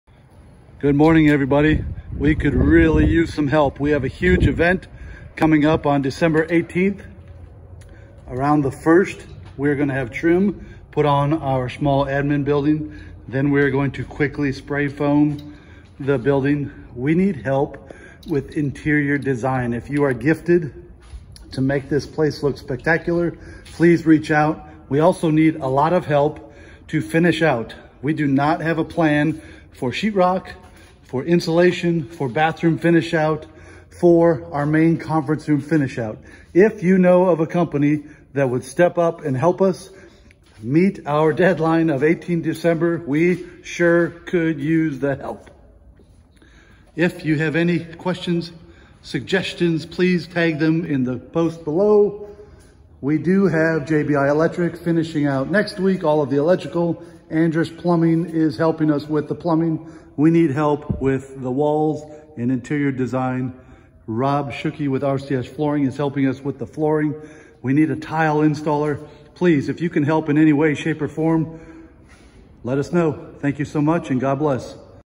HELP : Finish Dream Center Admin Building
We need your HELP to finish this out by 18 December — We are going to spray foam exterior walls (~$5,300) - we still need insulation for is the interior bathrooms

🙏 500 Sq ft of R19 16” on center wide for ceilings = 7 Bundles (Batts)
🙏 For walls we need R13 and approximately 25 rolls
🙏 Interior Designer to help us plan
✅ Flooring - RCS Flooring
🙏 Flooring installer
✅ Electrical - JBI Electrical
✅ Plumbing = Andress Plumbing